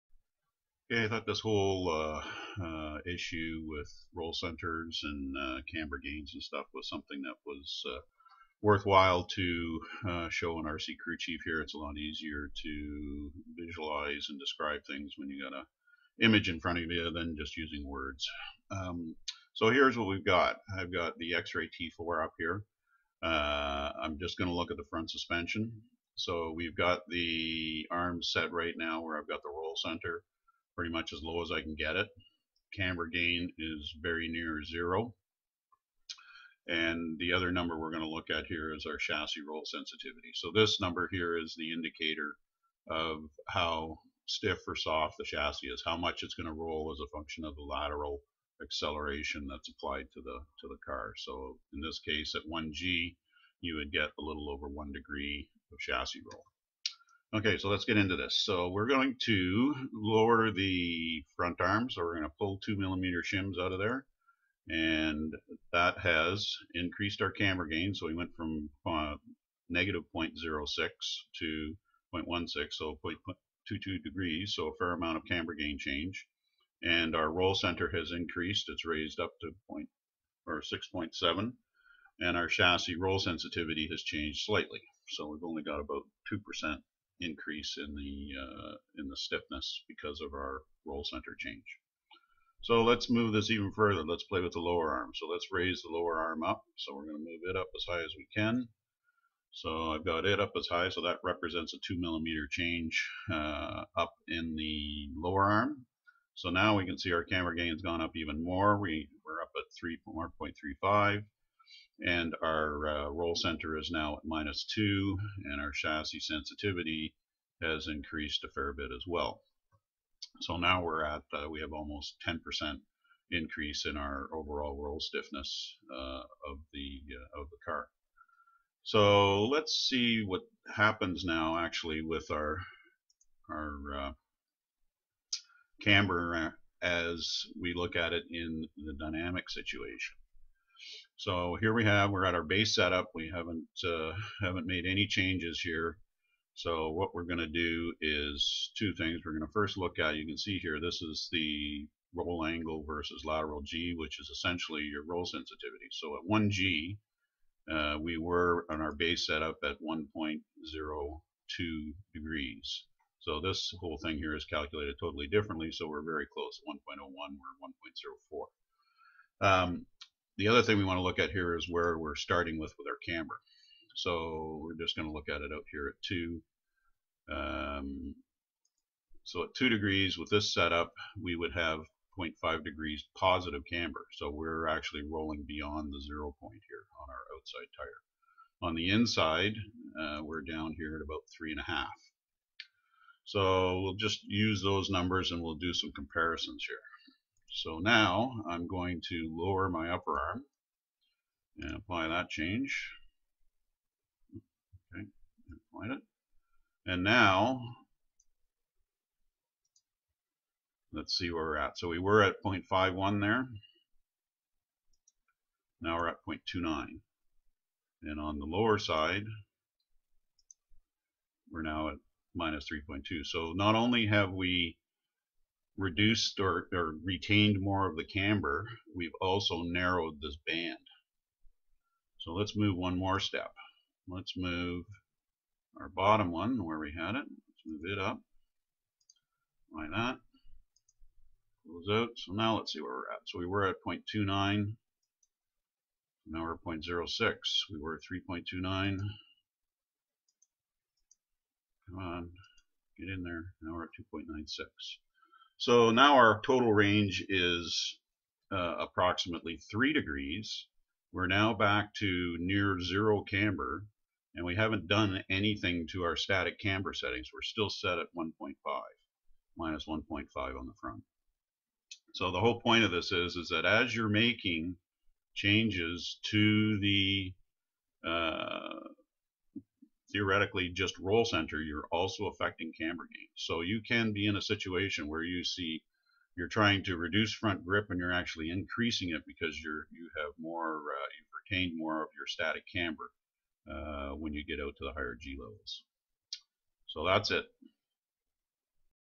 Roll Centre and Camber Gain
Effect of Roll Centre changes on Camber and Camber Gain